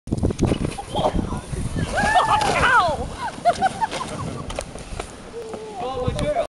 Shelley sledding
Sledding down the neighborhood church steps.